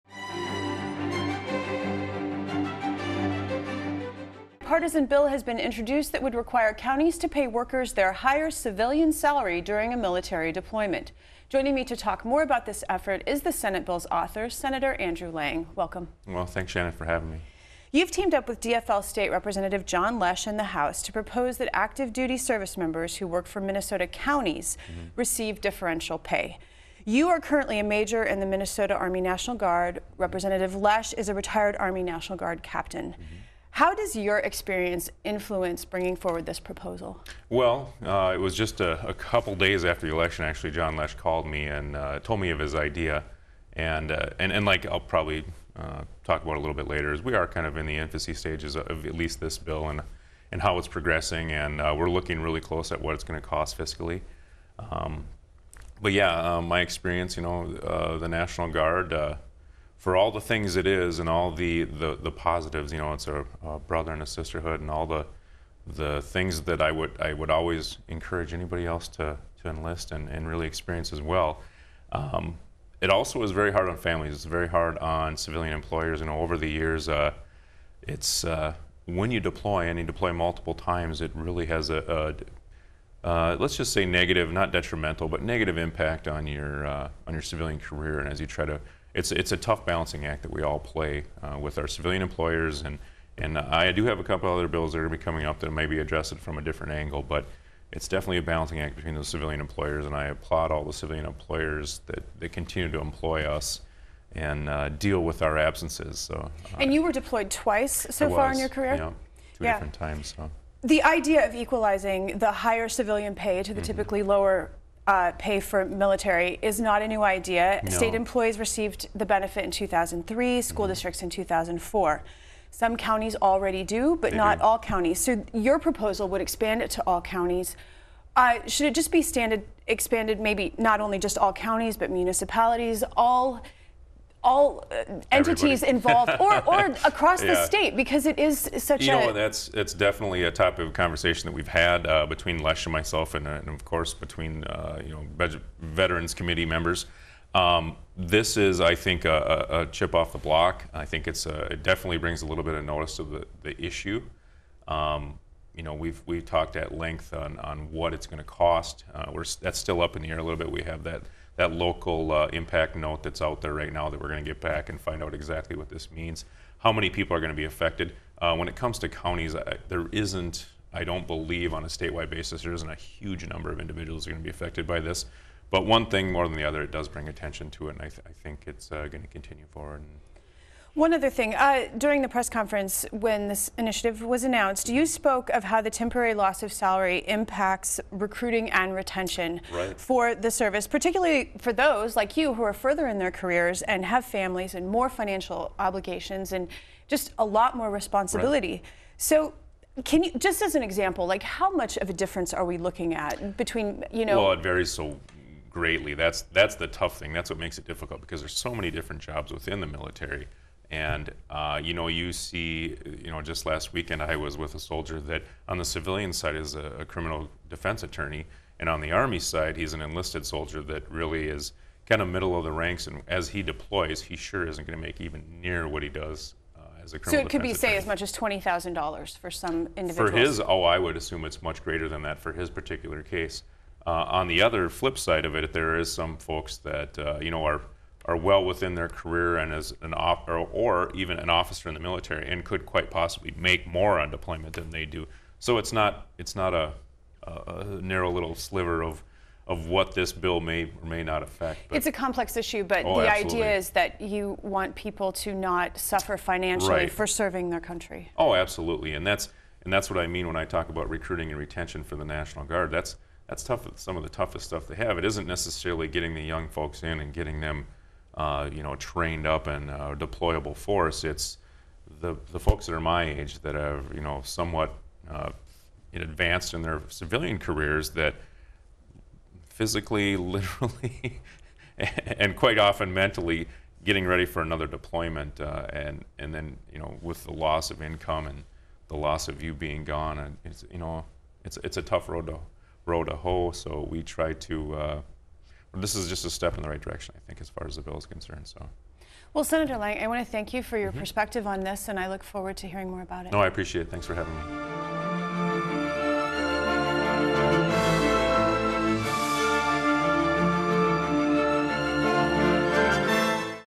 Providing Differential Pay for Soldiers
Senator Andrew Lang, R-Olivia, joins Capitol Report moderator Shannon Loehrke to talk about his bill that would require all counties to make up the difference between an employee's civilian salary and military salary during a military deployment.